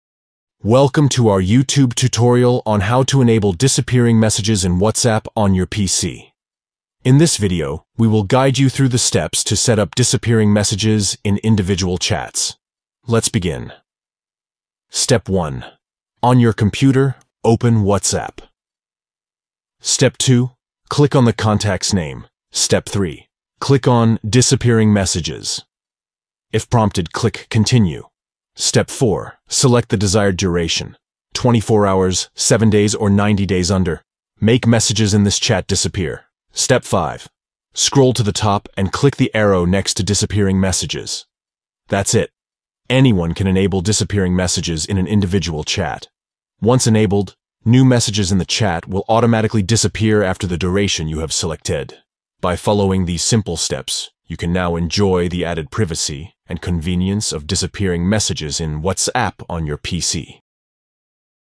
How To Enable Disappearing Messages In WhatsApp On PC
Learn how to enable disappearing messages in WhatsApp on your PC with this step-by-step tutorial:

1. On your computer, open WhatsApp.
2. Click the contact's name.
3. Click on "Disappearing Messages." If prompted, click "Continue."
4. Select the desired duration: 24 hours, 7 days, or 90 days under "Make messages in this chat disappear."
5. Scroll to the top and click the arrow next to "Disappearing messages."

Discover how to set up disappearing messages in WhatsApp on your PC. This tutorial will guide you through the steps to enable disappearing messages in individual chats.

Don't miss out on this informative tutorial that will help you take advantage of disappearing messages in WhatsApp on your PC.